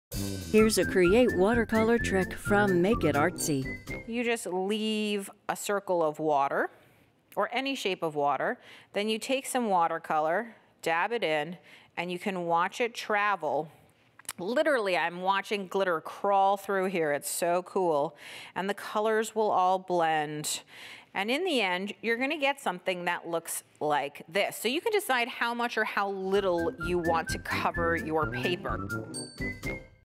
Create Quick Tip: Adding Glitter to Watercolors
Julie Fei-Fan Balzer of Make it Artsy explains how to add glitter to your watercolor palette.